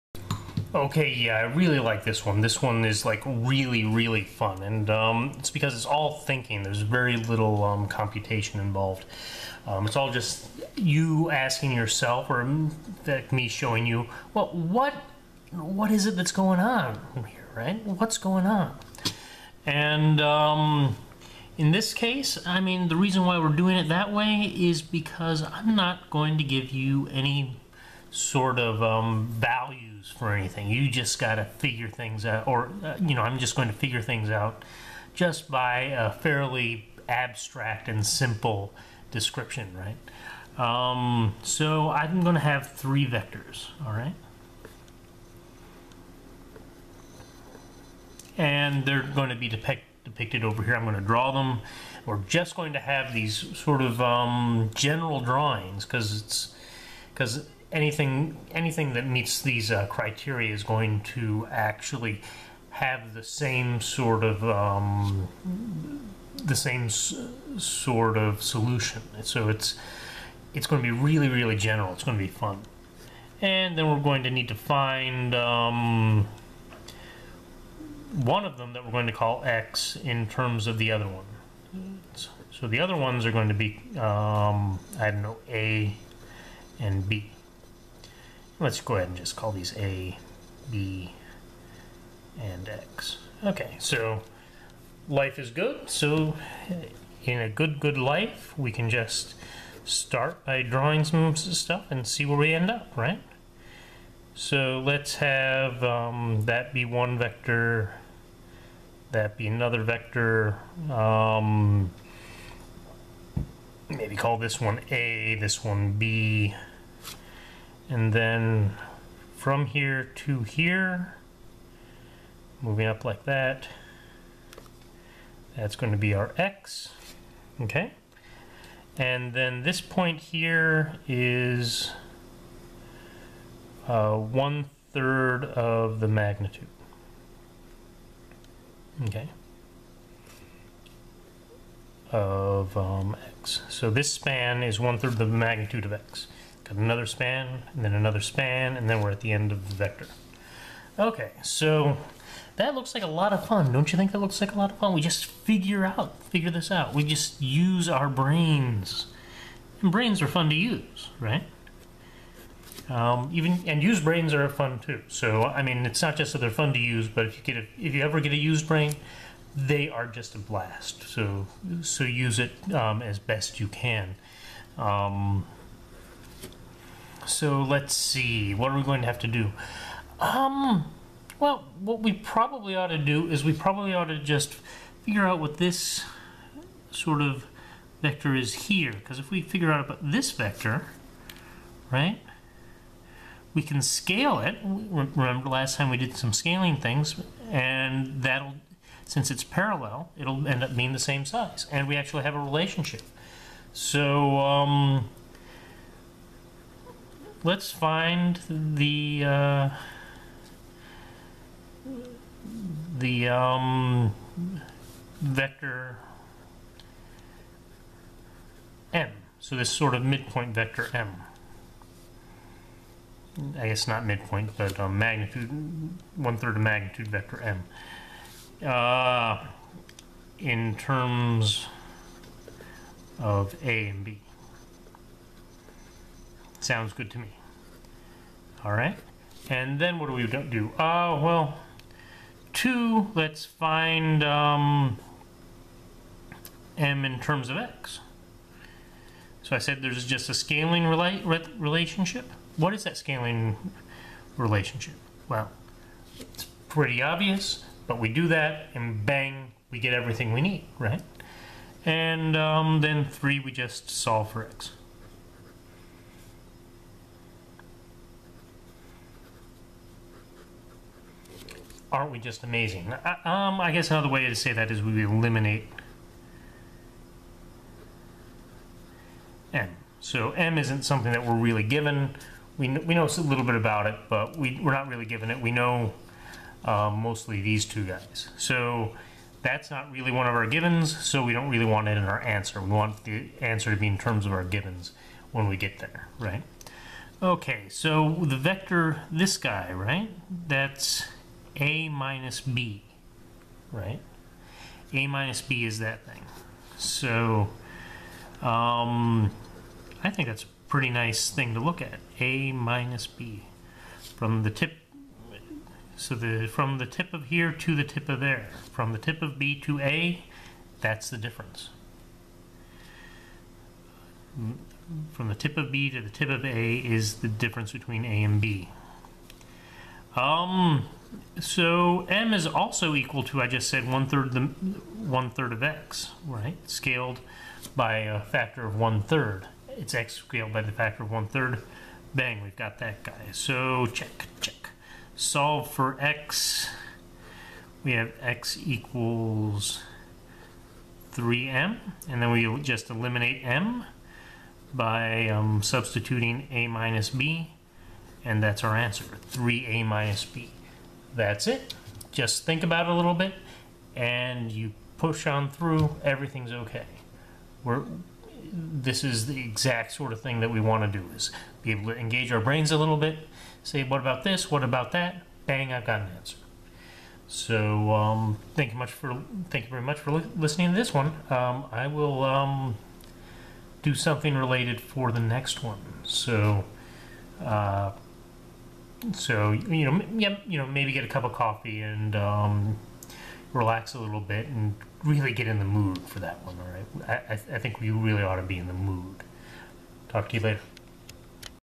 Finding One Vector in Terms of Two Others
This is a description of how to find the relationship between one vector and two others using a a qualitative reasoning process.